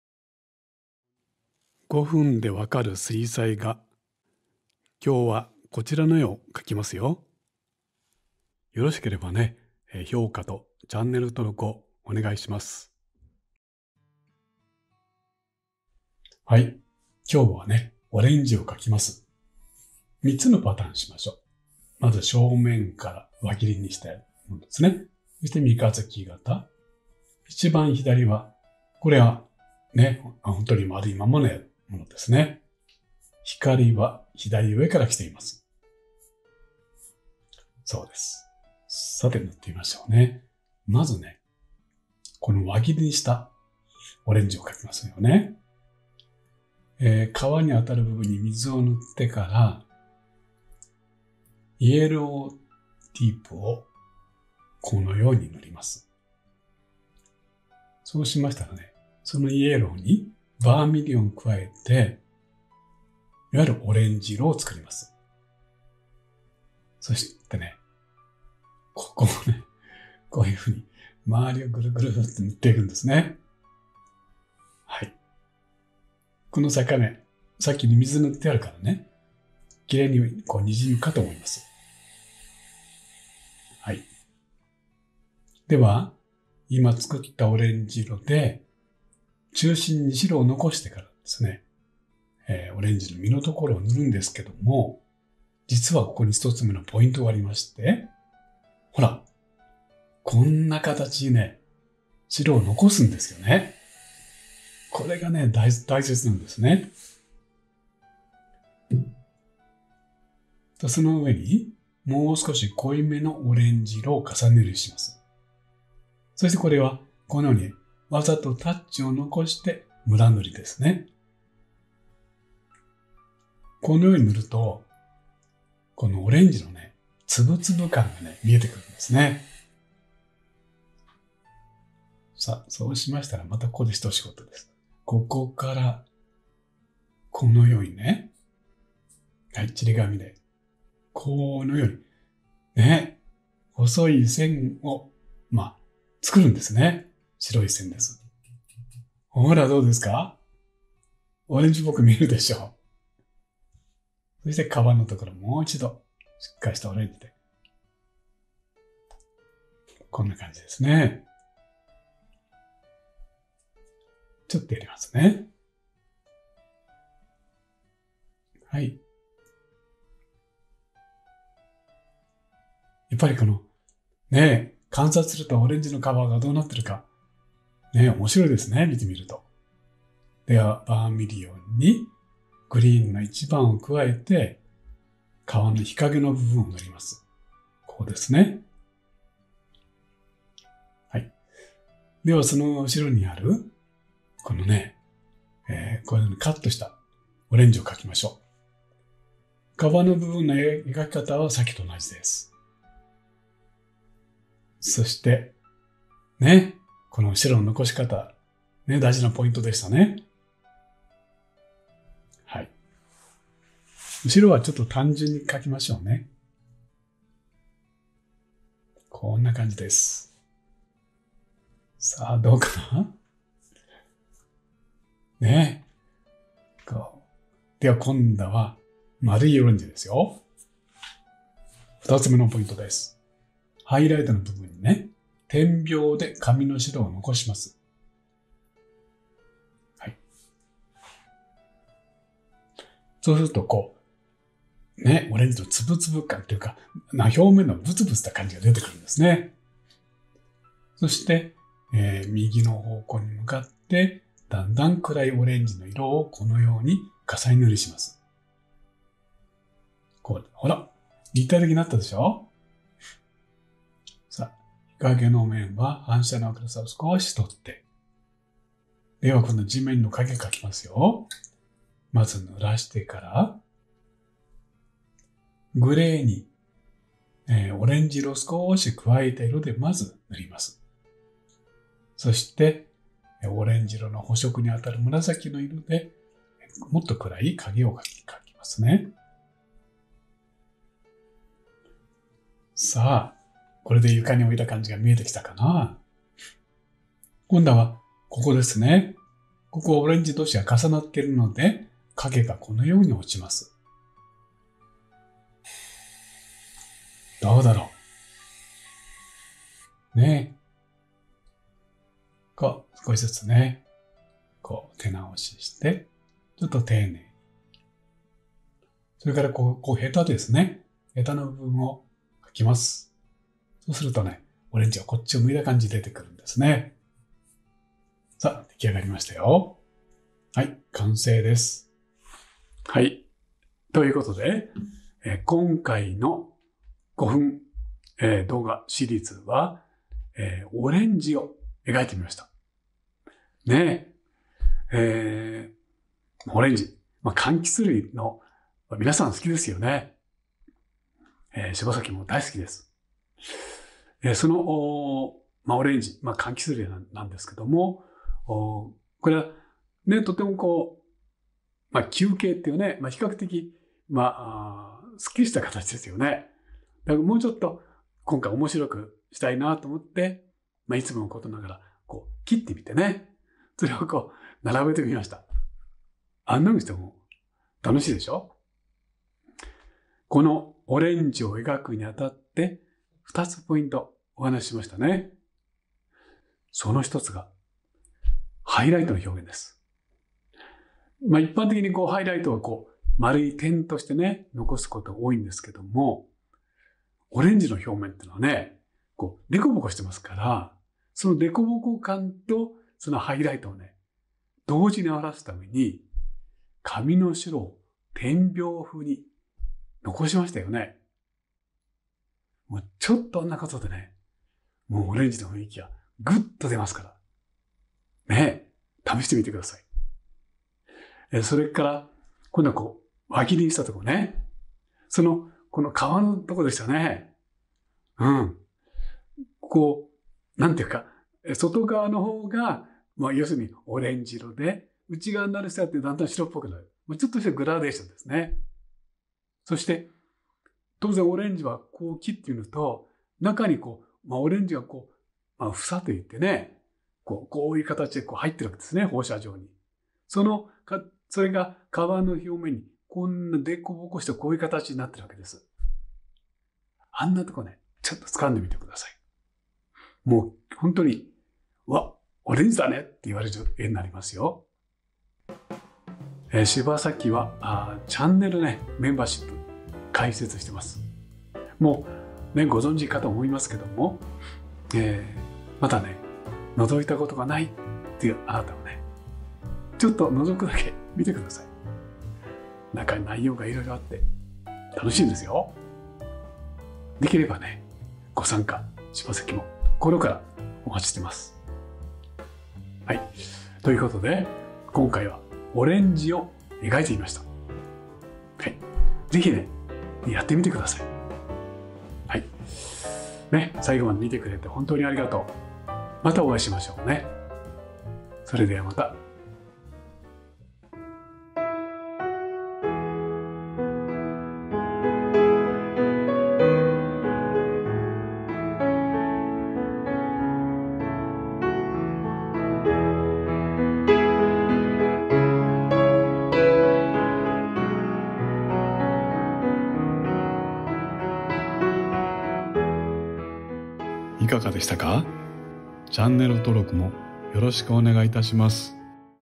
[Eng sub] How to draw the orange / 5 min Watercolor / Tutorial for beginners
This is Shibasaki, your watercolor instructor. Today, I will show you how to draw oranges in 5 minutes. Fruits are simple in shape and bright in color, so if you are a beginner, I recommend starting with this drawing of a fruit. By understanding just two points, you can draw an amazingly realistic orange, so please watch this video and try to imitate it.

Please send the fan letter to this address☟
Harumichi Shibasaki
P.O.Box #1
Isumi Post Office
Chibaken　298-0199
JAPAN